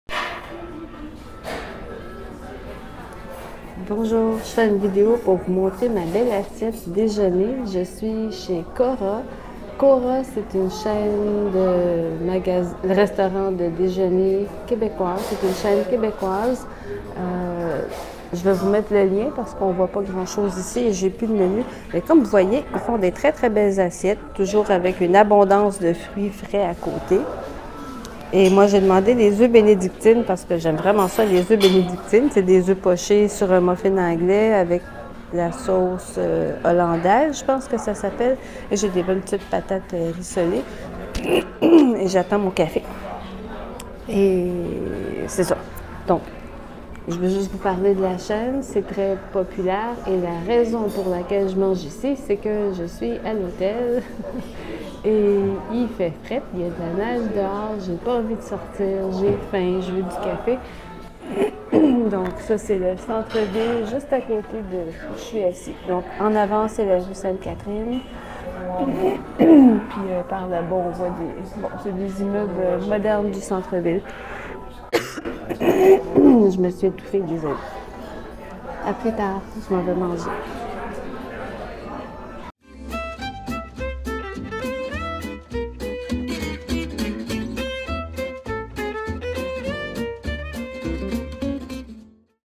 LE RESTAURANT CORA À MONTRÉAL - Intermediate Quebec French with Subtitles | Frenchpresso
Version courte :

Voici mon déjeuner au restaurant Cora, qui est une chaîne de restaurants très populaire au Québec. J'ai mangé des oeufs bénédictine. C'était délicieux!

Version longue :

Quand je suis arrivée à Montréal, le 28 février, après un long voyage et une superbe escale à Munich, j'étais tellement fatiguée que je suis allée me coucher sans souper... Le lendemain matin, j'avais très faim! Mais comme il faisait froid, j'ai été paresseuse et j'ai tout simplement mangé dans le restaurant situé au rez-de-chaussée de mon hôtel, que je vous ai montré mercredi :

Cora, c'est une chaîne québécoise très populaire, qui a été fondée en 1987. C'est Cora, la fondatrice de la chaîne, qui a inventé le concept de ces restaurants de déjeuners, qui sont maintenant très populaires - il y a eu plusieurs imitateurs! Voici quelques liens, si ça vous intéresse :

J'aime vraiment les oeufs bénédictine! J'ai déjà fait une autre vidéo à Montréal où je montre mes oeufs bénédictine :

En terminant, voici quelques notes de prononciation et de vocabulaire familier québécois, avec des liens aux explications que j'ai déjà données :

j'ai pu de menu = je n'ai plus de menu